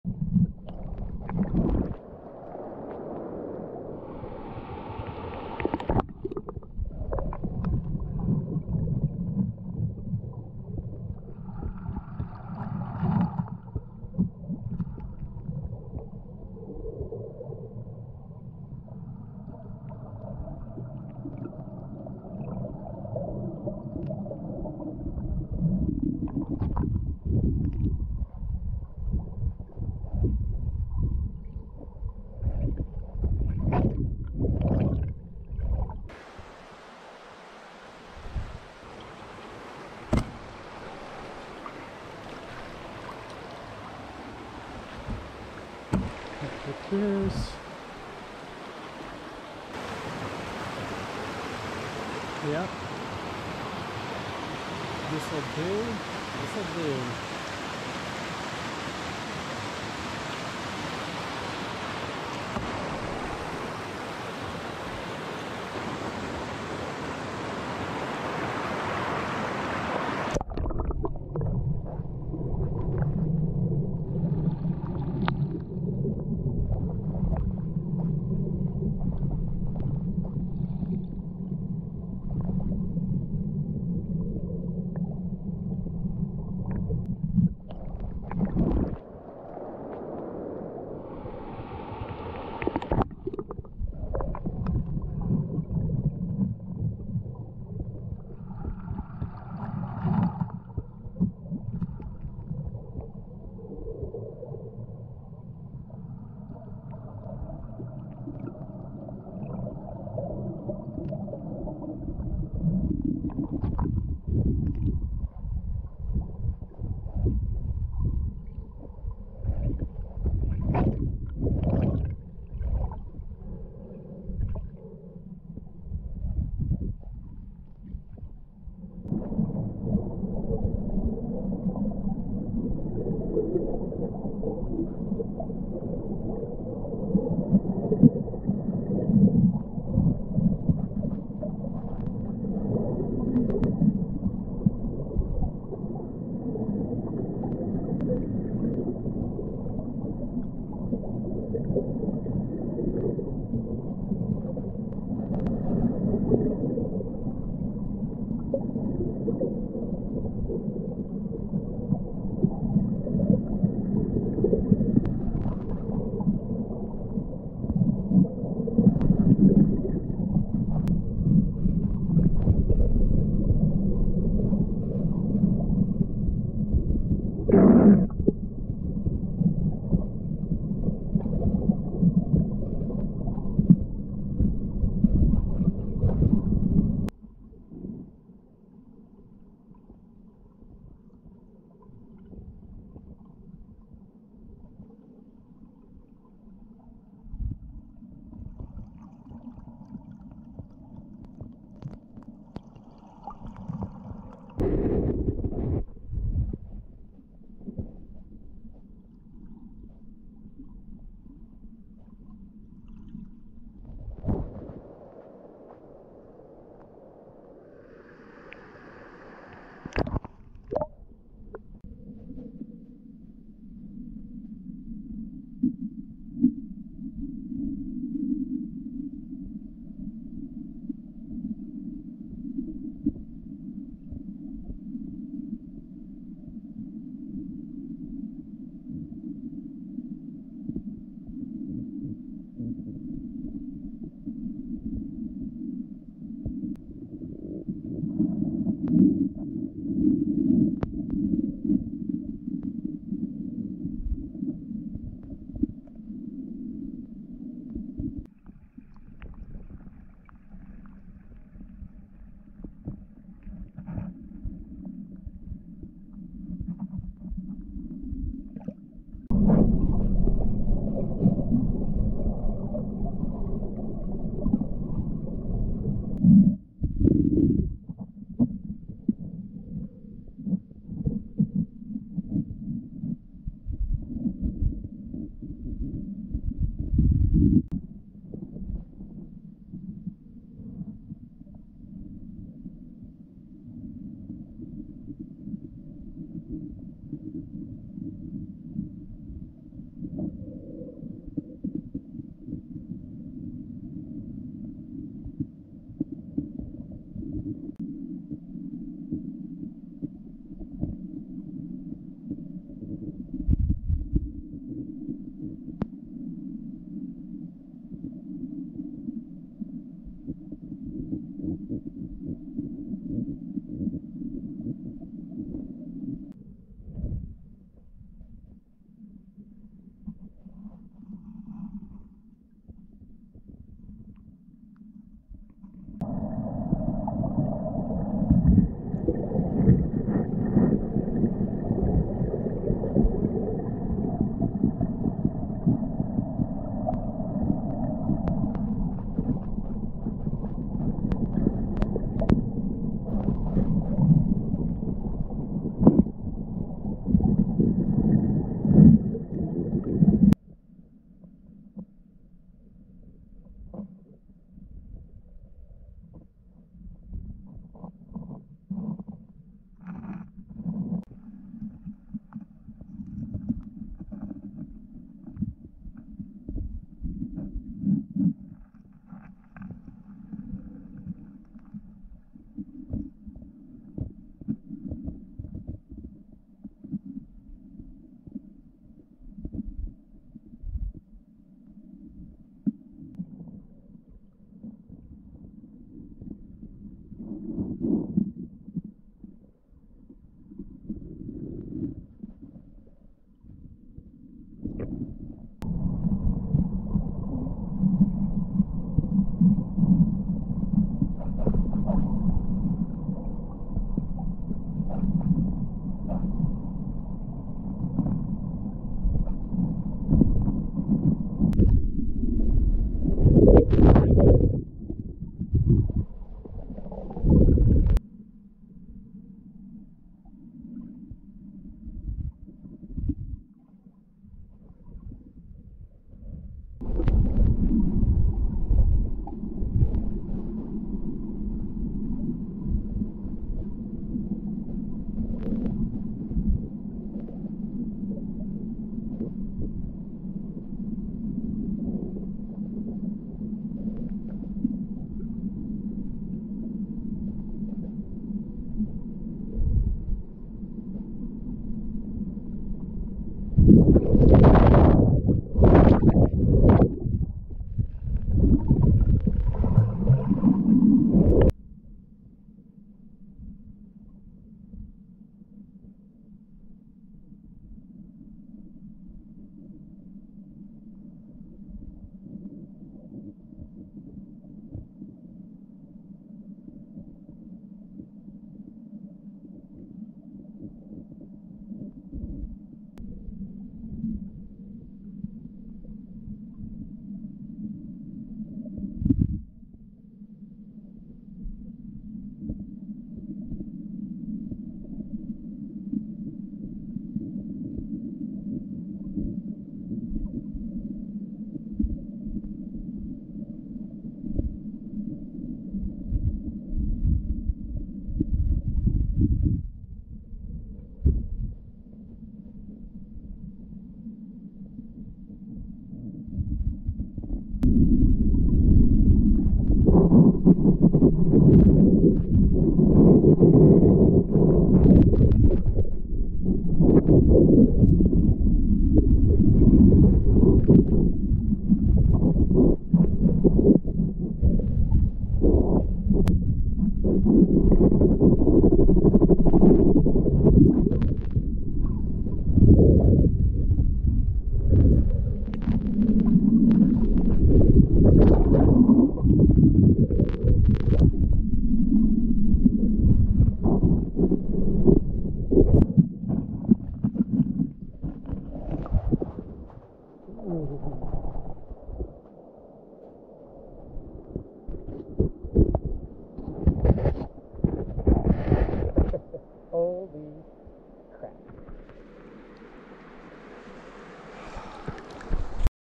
Field Study - Tremont Snorkeling Hole
In September Dylan and I went on a week-long snorkeling adventure to explore the rivers in Cherokee National Forest. I began the adventure by attending a Naturalist Skills course at The Great Smoky Mountain Institute at Tremont. This video is from the field study that I completed during this course. The goal of the field study was to spend time observing nature with a question in mind. My question was pretty simple: What's under the water?

3:07 - Silver Shiner
3:41 - Warpaint Shiner
4:47 - River Chub
6:00 - Northern Hog Sucker
6:33 - Tennessee Snubnose Darter
6:52 - Smallmouth bass (juvenile)
7:01 - Rainbow Trout
7:39 - Bass (either Smallmouth, Largemouth, or Spotted. My best guess is Smallmouth)
8:47 - Aquanaut
9:18 - Cardinal flower